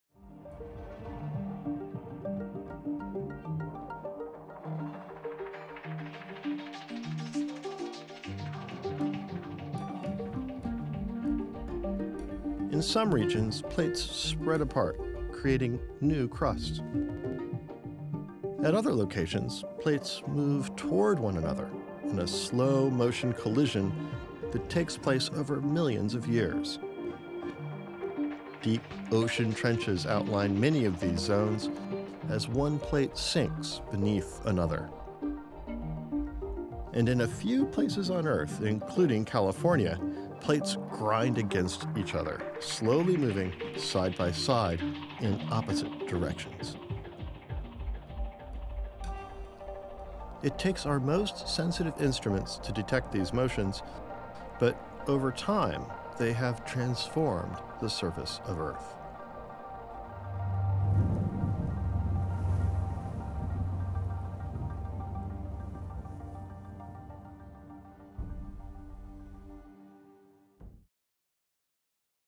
Plate Boundaries: Divergent, Convergent, and Transform | California Academy of Sciences
Although the ground beneath us seems solid, it is actually in constant motion. Movement in narrow zones along tectonic plate boundaries causes most earthquakes. Most seismic activity occurs at three types of plate boundaries – divergent, convergent, and transform.

For more on earthquakes—why they happen, how they’ve shaped the Bay Area, and what you can do to prepare for future events